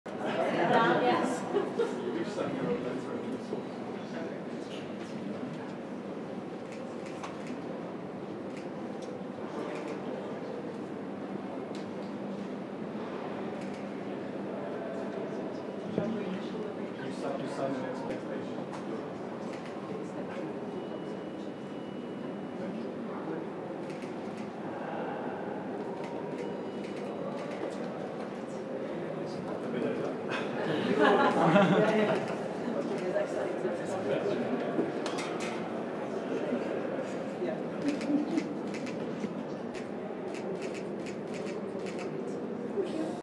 Ue, Roberta Metsola incontra Christine Lagarde al Parlamento europeo
La presidente della Bce prima dell'audizione in commissione Econ

 (Bruxelles). La presidente del Parlamento europeo Roberta Metsola ha incontrato la presidente della Banca centrale europea (Bce) Christine Lagarde, prima della sua audizione in commissione parlamentare per gli Affari economici e monetari (Econ). Con loro, la presidente della Commissione parlamentare, l'eurodeputata del Partito democratico Irene Tinagli. (Federica Onnis/alanews)